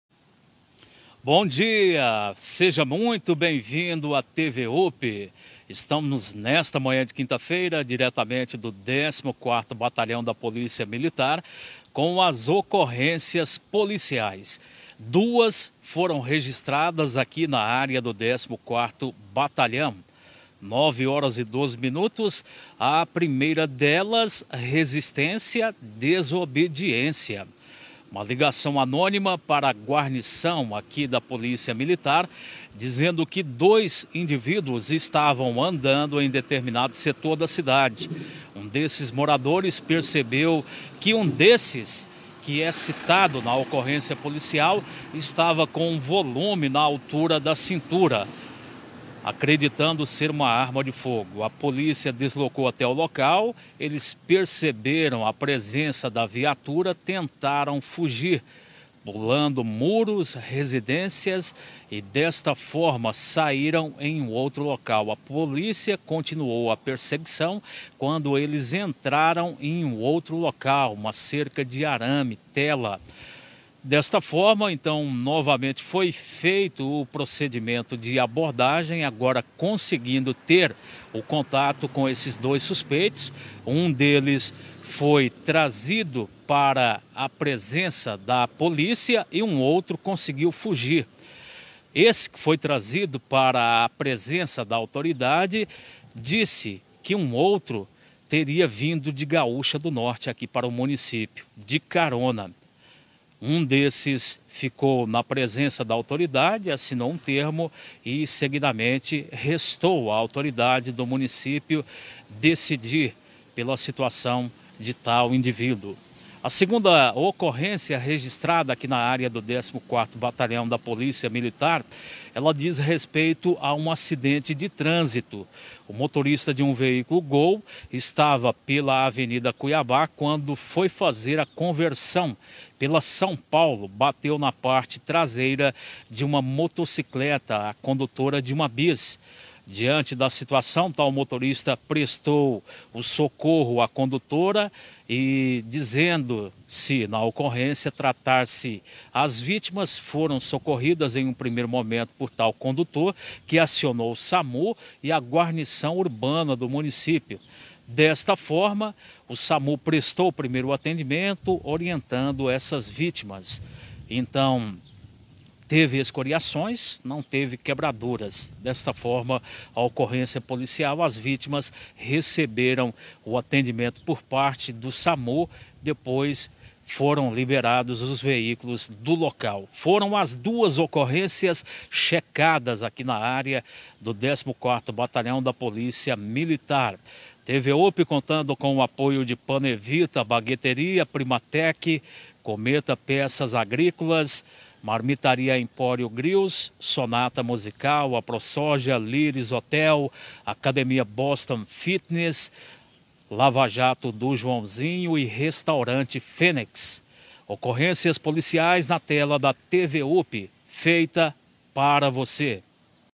Boletim de ocorrência das últimas 24hrs de Primavera do Leste-MT. 06/02/2020
Boletim de ocorrência das últimas 24hrs com o trabalho da Polícia Militar de Primavera do Leste-MT. 06/02/2020